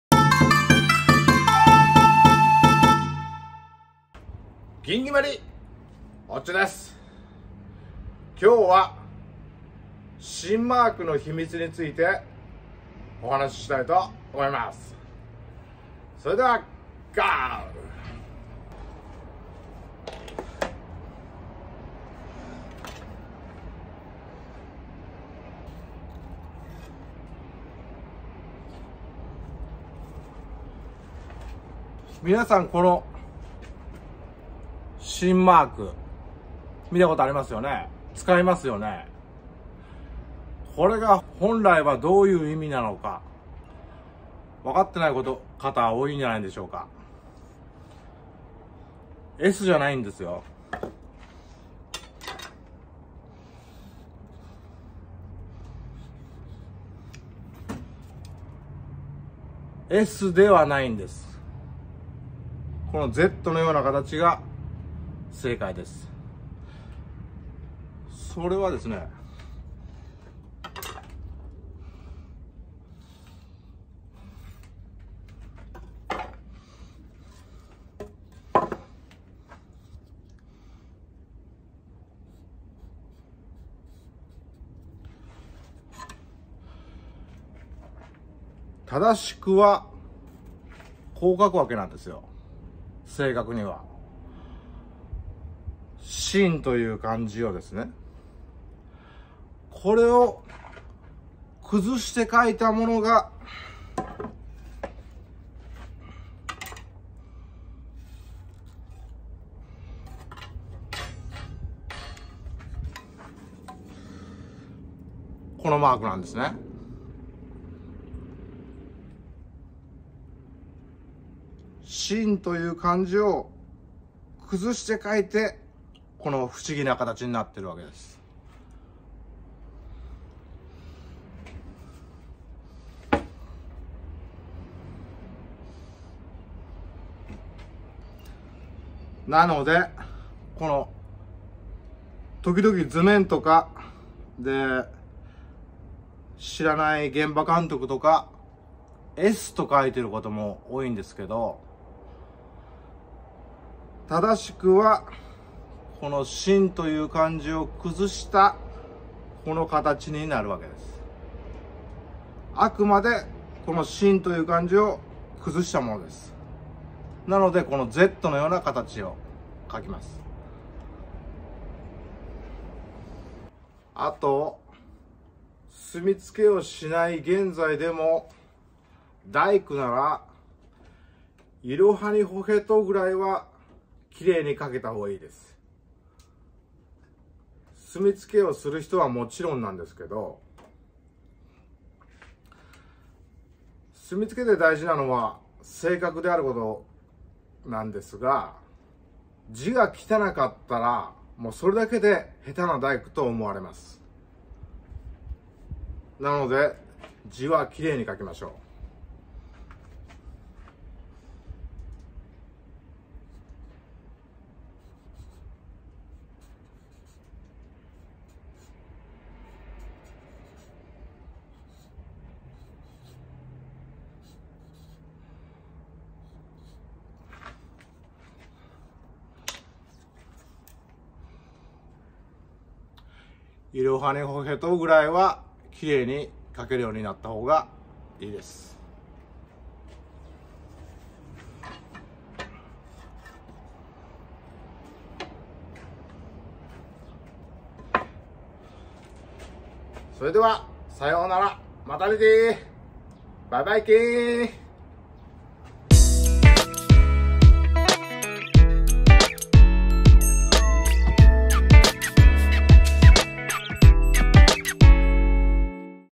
【日本の大工の技】芯マークの秘密を教えます！※この情報は諸説あり！真実を知ってる方、是非コメント下さい！！！！！！Japanese carpenter skills
みなさん日頃よく目にする芯マーク、その秘密を語ります！
※この情報はあくまでボクの長年の見聞きした情報をもとにした確信のないモノです
真実を知ってる方、是非コメント下さい！！！